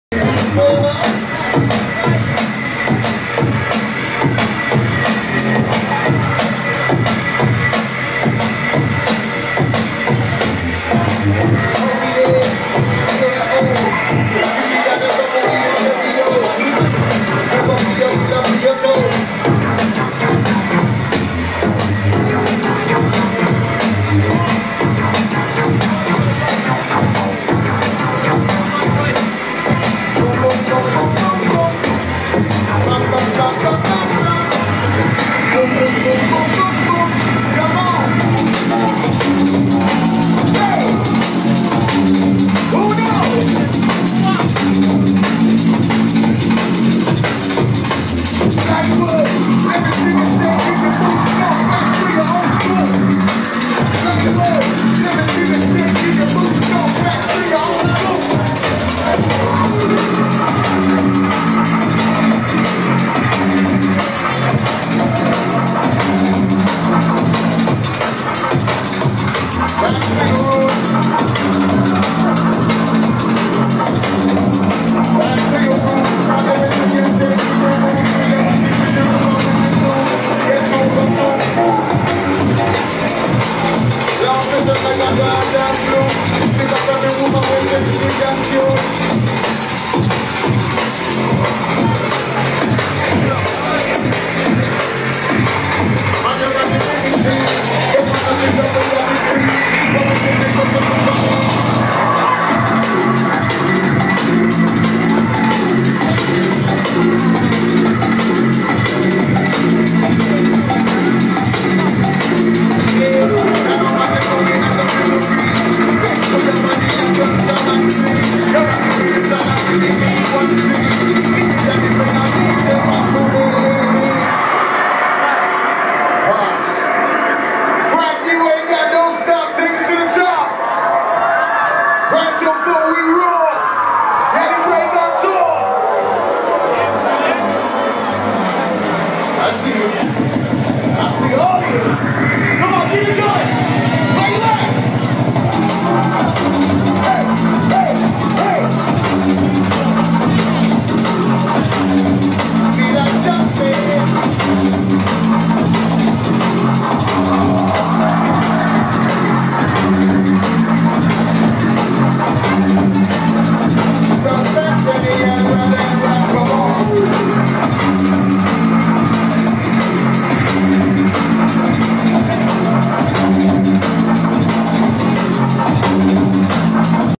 Hospitality Brighton @ Digital 9 Oct 2010
Hospitality Brighton 5th birthday at Digital with London Elektricity & Danny Byrd